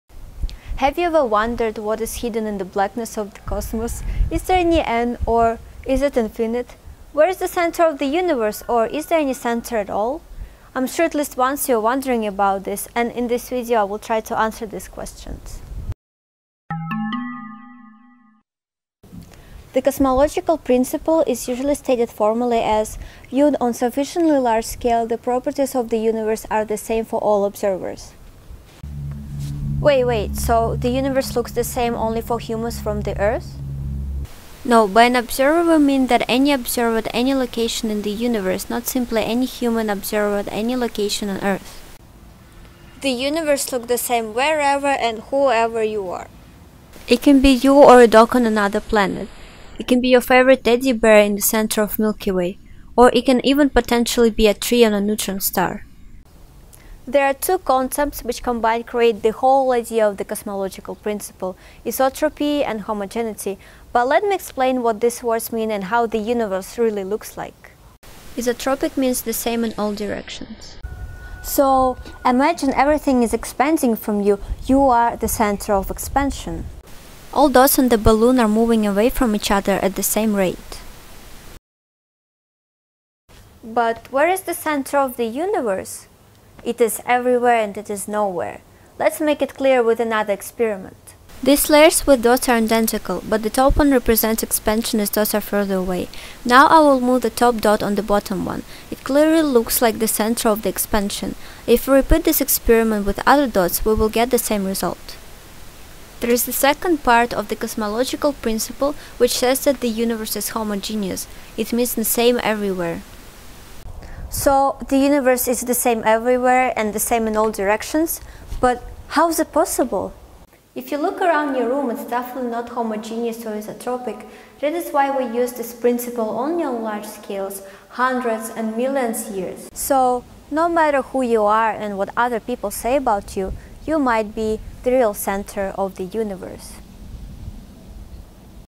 Cosmological Principle | Breakthrough Junior Challenge 2018
Have you ever wondered where you live and what is hidden in the blackness of the cosmos? Is there any end or is it infinite? Where is the center of the universe? ...or is there no center?
I am sure at least once you were thinking about it. In this video I will try to answer these questions.

This video was made for breakthroughjuniorchallenge